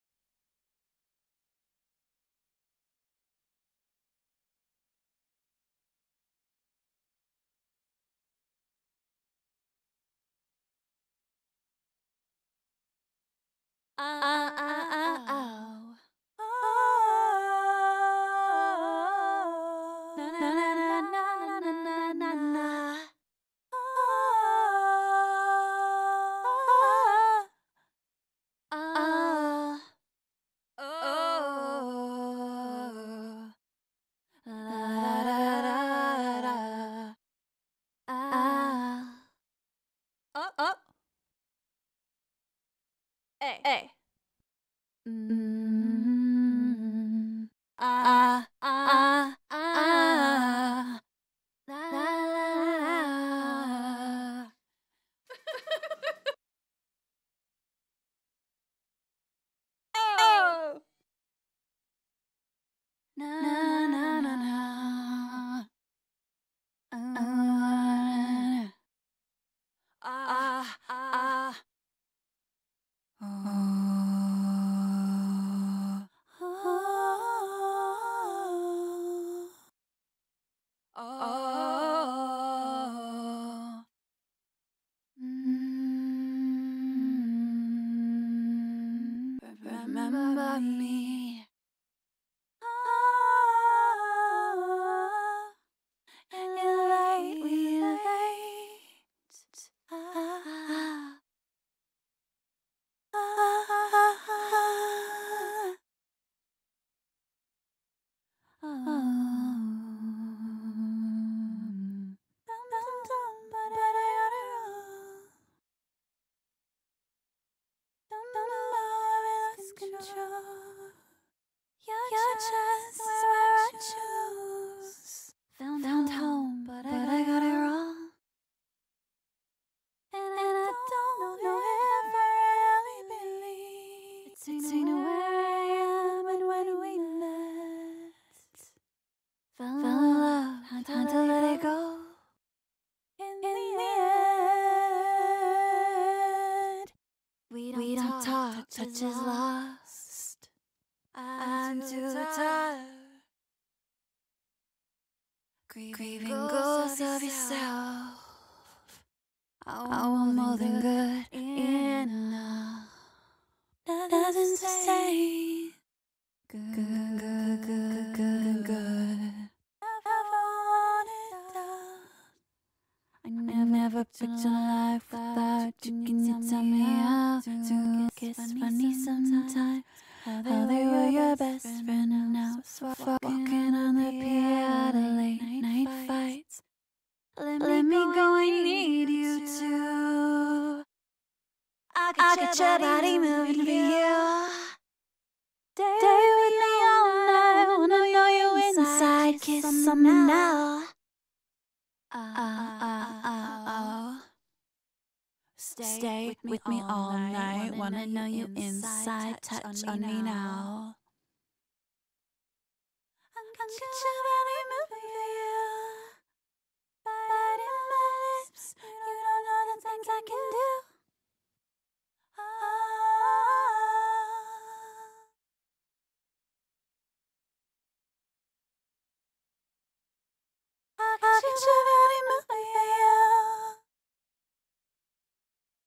Cubase 11 Pro | VST Sounds | Bloom preview
Amy Kirkpatrick's vocals are the content for Bloom, which provides a wide range of vocal phrases and one shots for your music production.  Have a listen in this video overview of the content pack.